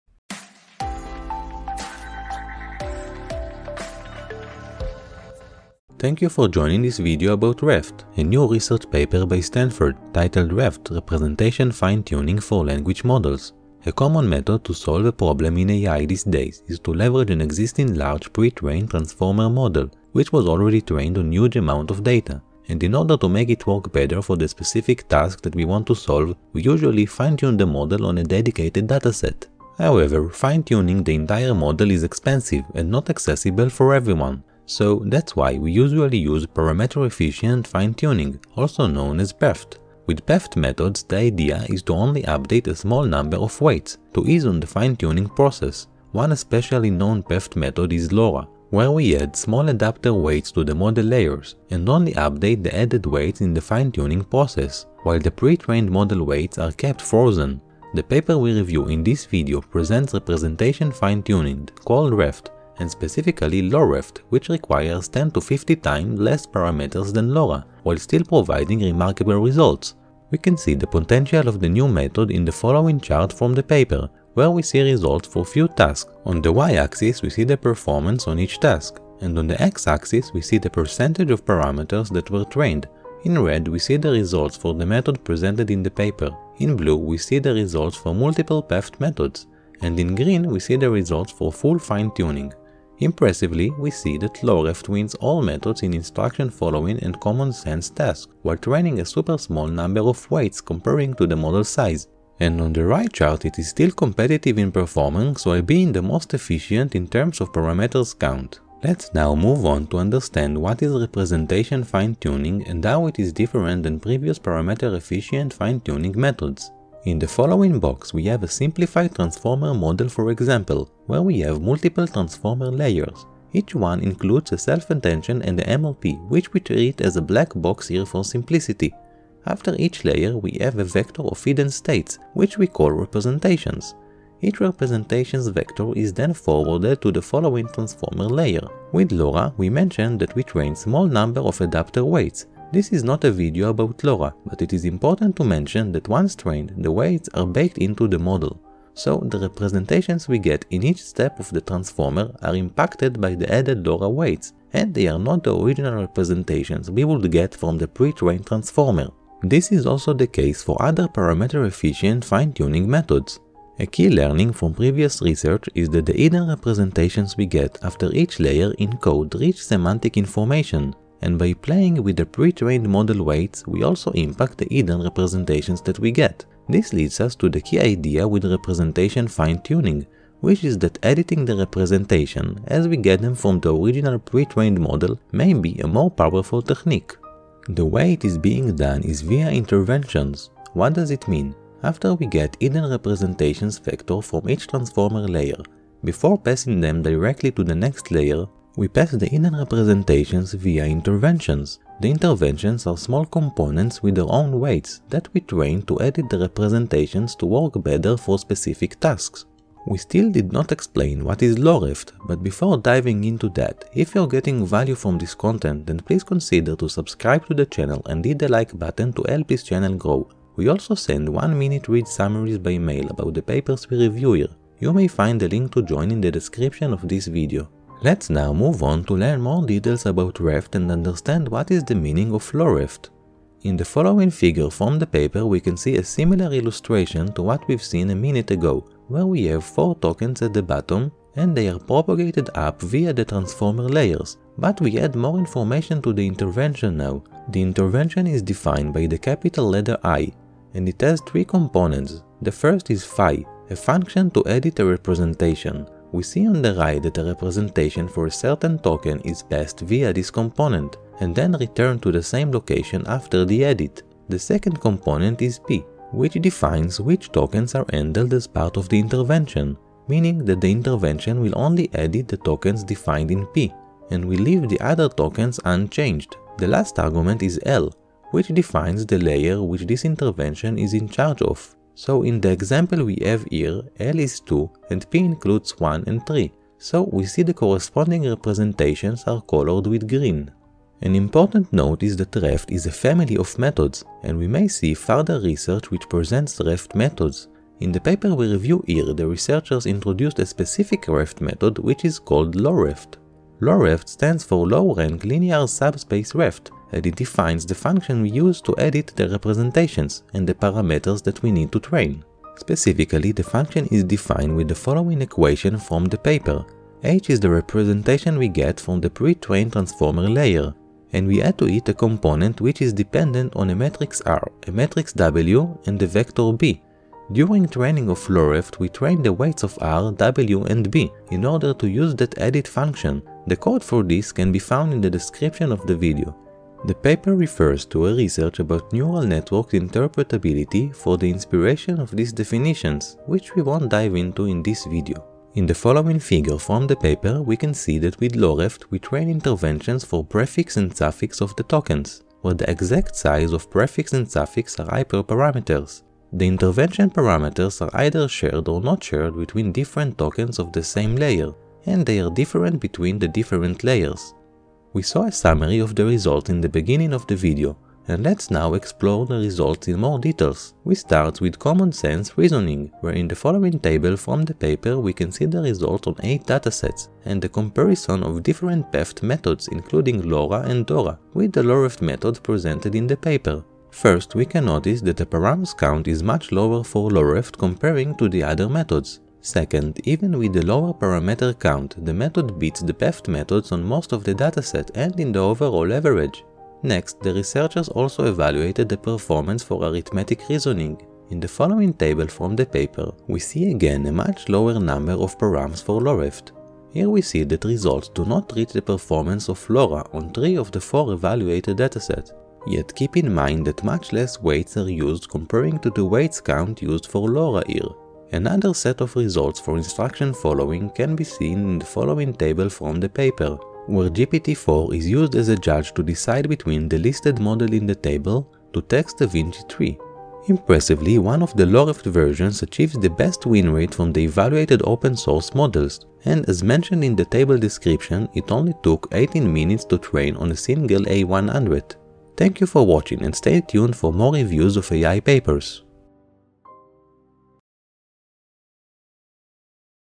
ReFT: Representation Finetuning for Language Models | AI Paper Explained
Can LoReFT be a rival for LoRA? According to ReFT paper, it has the potential to replace LoRA in various cases.

In this video we dive into the research paper that presents ReFT and LoReFT. We'll explain what is representation fine-tuning (ReFT), and how it is different than previous parameter-efficient fine-tuning (PEFT) methods, such as LoRA.
ReFT is a family of methods that can be used to adapt pre-trained transformer models to specific tasks, and we'll understand the concept of how such methods work. Specifically, the paper presents a concrete ReFT method which is called LoReFT, which stands for Low-rank Linear Subspace ReFT. We'll explain how it works, and see results from the paper that show the great potential of this method comparing to previous PEFT methods.

Chapters:
0:00 Introduction & Motivation
1:49 What is ReFT?
3:47 ReFT & LoReFT Details
6:07 LoReFT Results